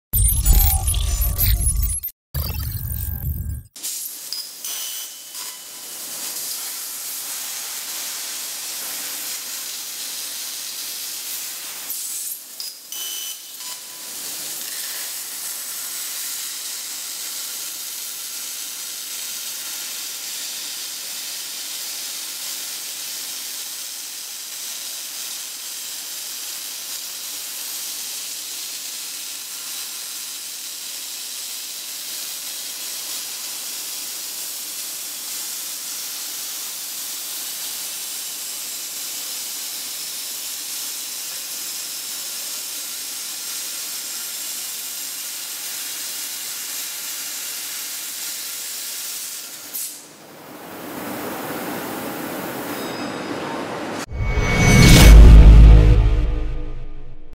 15 KW Laser Cutting - 40mm Stainless Steel
Kind Reminder: you are watching 40mm Stainless Steel laser cutting demo:

Han's Laser Smart Equipment Group Co., Ltd launched 15kw High Power Fiber Laser Machine Series with IPG laser source. The High Power Laser Cutting is now becoming a trend for thick metal plate processing.